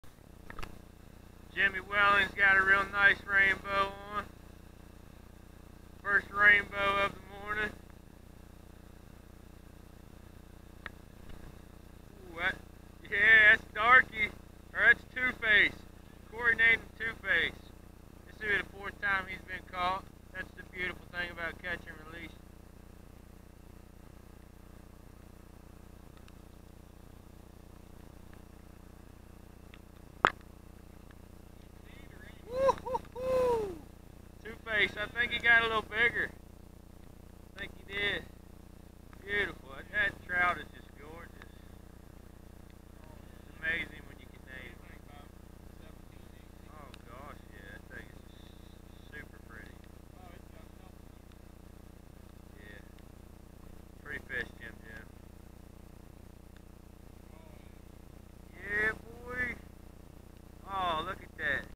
Jimmy Wellings catching TWO FACE for the 2nd time.
Cold Rainy Day in Feb 2017. River Conditions High and murky. Western MD North Branch Potomac River Trout Fishing